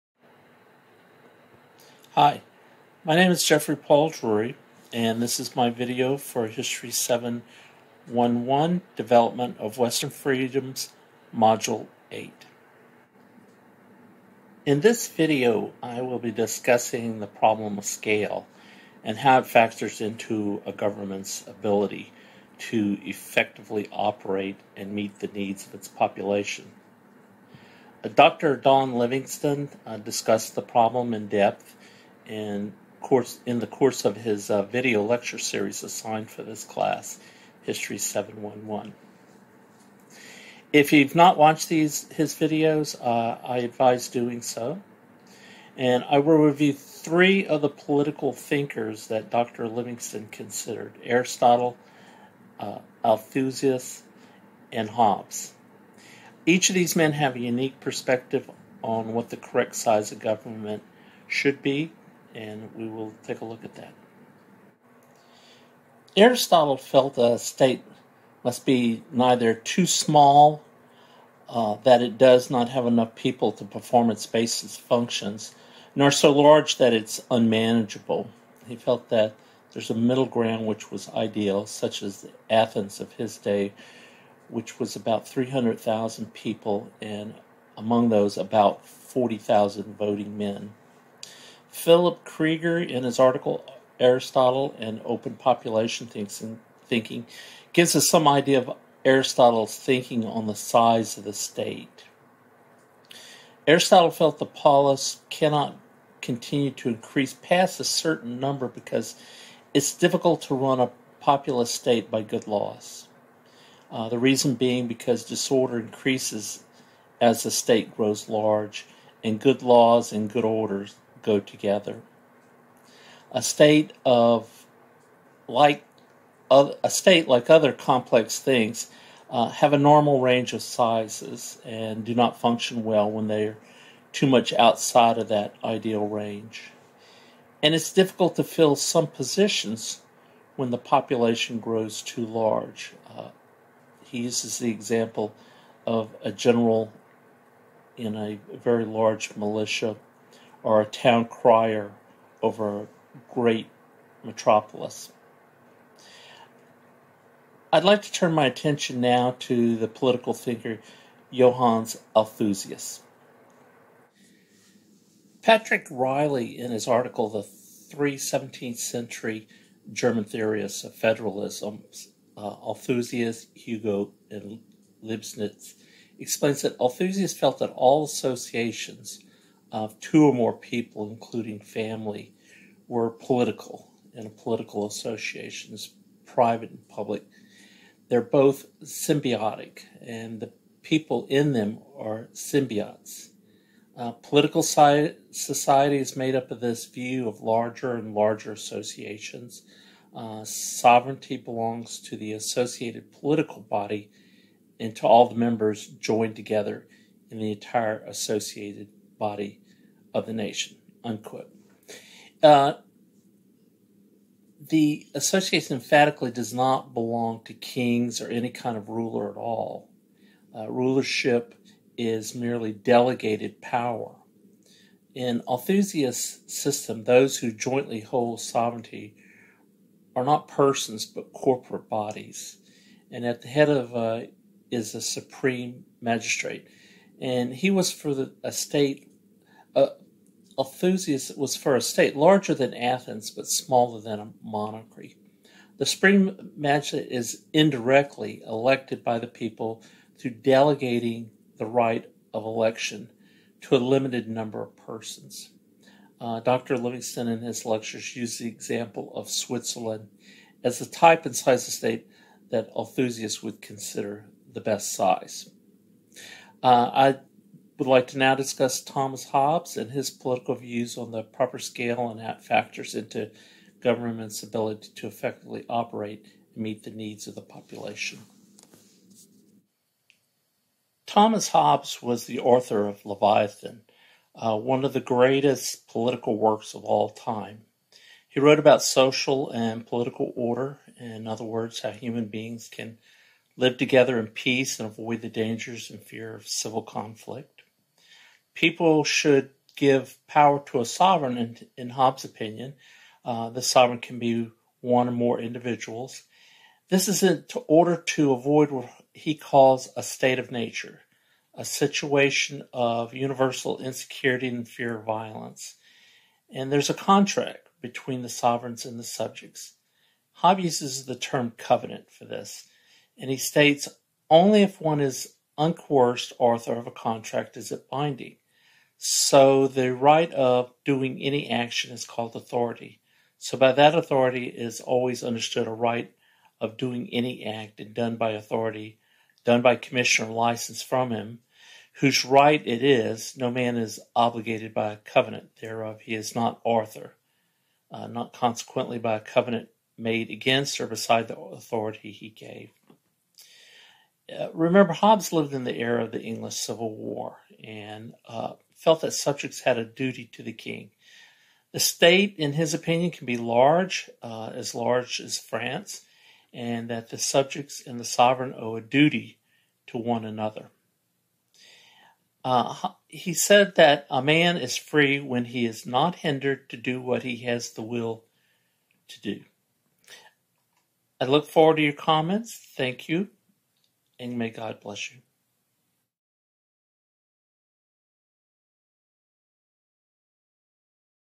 The problem of the proper size of scale and function for effective government
The problem of the proper size of scale and function for effective government is one which a great many political thinkers have considered over the ages. Dr. Don Livingston discusses this issue in his lectures on the subject for History 711. He discusses three great thinkers who have considered the problem: Aristotle, Althusius and Hobbs. Aristotle felt that government should be large enough satisfy the satisfy the political order. Althusius felt that the state should be smaller than a monarchy, such as Switzerland. Hobbes allowed for a larger government which functioned as artificial limbs for the King. This video will look at each in turn.

Bibliography
Hobbes, Thomas. The Elements of Law, Natural and Politic: Part I, Human Nature, Part II, de Corpore Politico; With Three Lives. Oxford University Press, USA, 1999.
Livingston, Don. Althusius date unknow, History 711 lecture,
Kreager, Philip. “Aristotle and Open Population Thinking.” Population and Development Review 34, no. 4 (2008): 599–629.
Riley, Patrick. “Three 17th Century German Theorists of Federalism: Althusius, Hugo and Leibniz.” Publius 6, no. 3 (1976): 7–41.
van Mill, David. “Hobbes’s Theories of Freedom.” The Journal of Politics, vol. 57, no. 2, [University of Chicago Press, Southern Political Science Association], 1995, pp. 443–59,